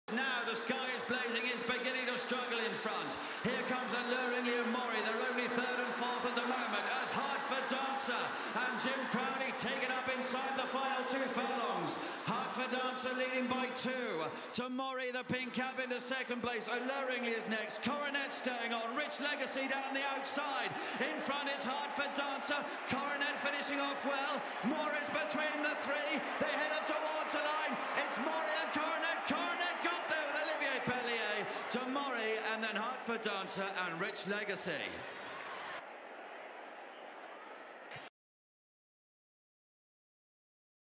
Royal Ascot 2017 | Coronet wins the Ribblesdale Stakes
Coronet, trained by John Gosden and ridden by Olivier Peslier, wins the Ribblesdale Stakes on Gold Cup Day at Royal Ascot.